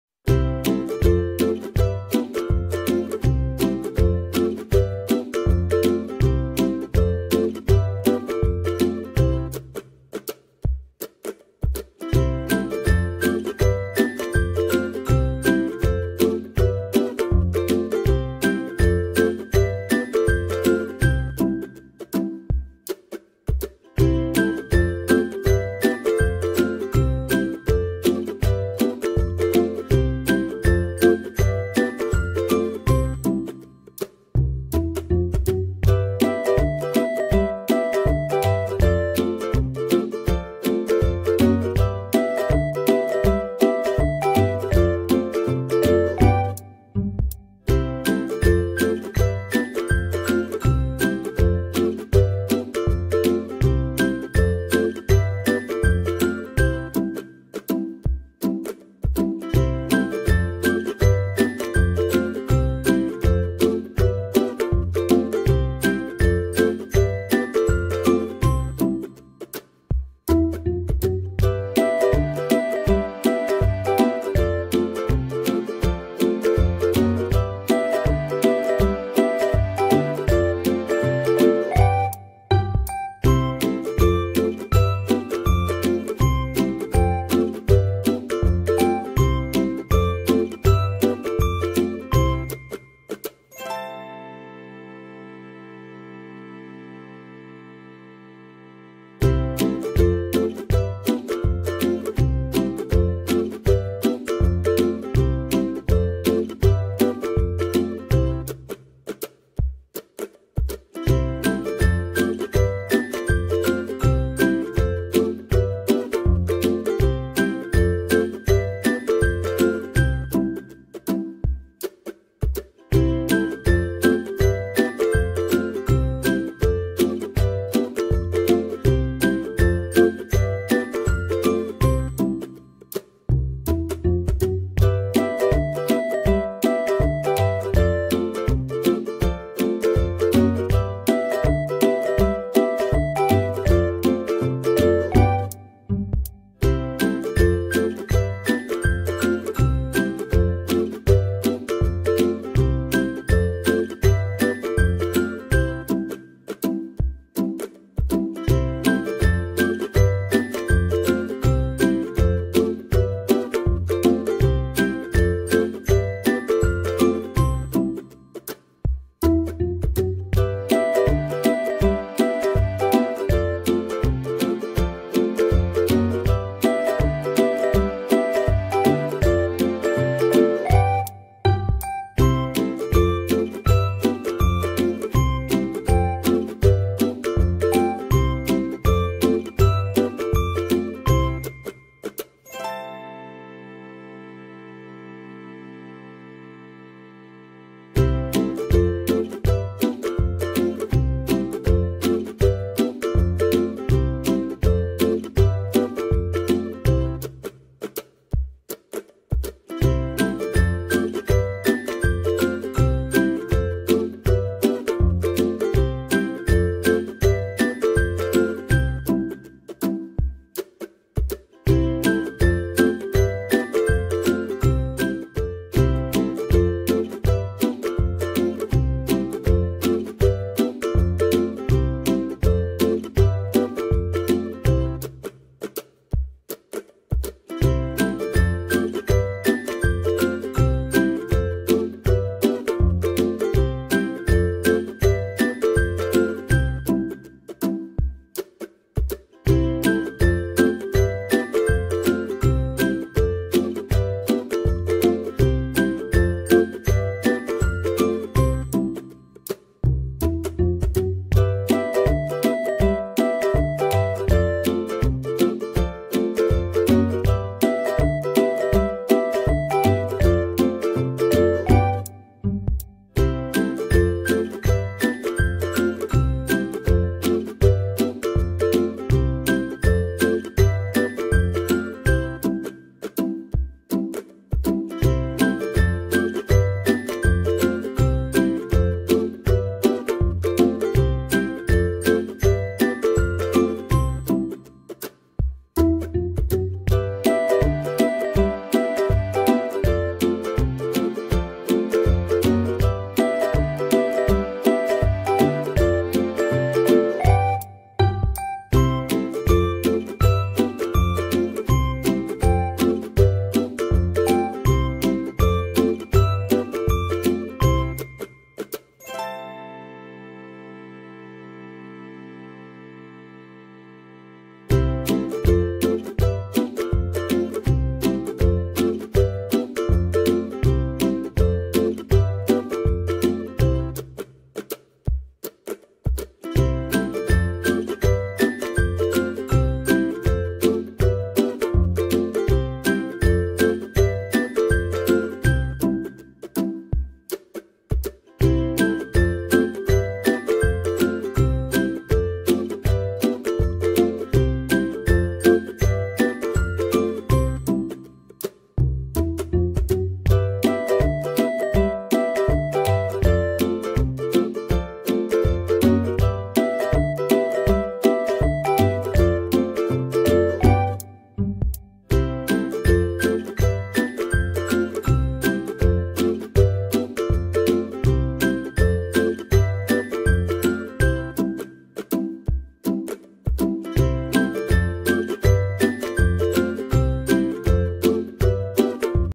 MENU DIET EXTREME NO GULA, NO TEPUNG, NO MINYAK, NO NASI.
Saya Tidak Menyarankan Menu Diet ini Untuk Jangka Panjang, Kalau Mau Mencoba Cukup 1,2,3 Hari saja. Setelah Itu Kembali Lagi Dengan Deficit Kalori Harian Kalian .